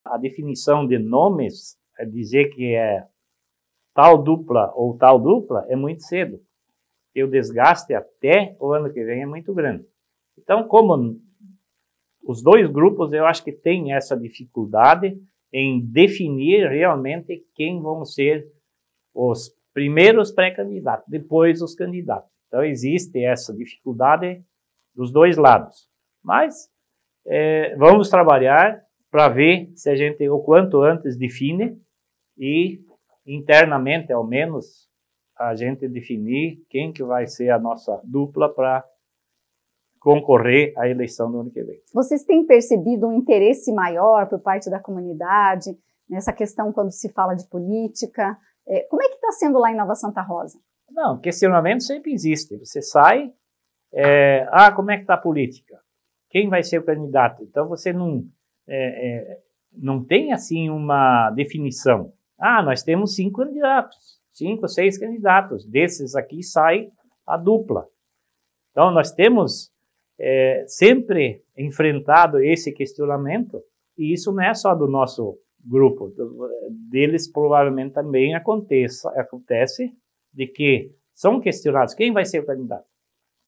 “𝐕𝐚𝐦𝐨𝐬 𝐭𝐫𝐚𝐛𝐚𝐥𝐡𝐚𝐫 𝐩𝐚𝐫𝐚 𝐝𝐞𝐟𝐢𝐧𝐢𝐫 𝐨 𝐪𝐮𝐚𝐧𝐭𝐨 𝐚𝐧𝐭𝐞𝐬”
Ex-vice-prefeito e ex-vereador de Nova Santa Rosa Lari Hitz fala sobre a indefinição dos nomes à majoritária, tanto por parte da situação quanto da oposição, e destaca um envolvimento maior da comunidade nas questões políticas.

Acesse O Presente e fique bem informado: opresente.com.br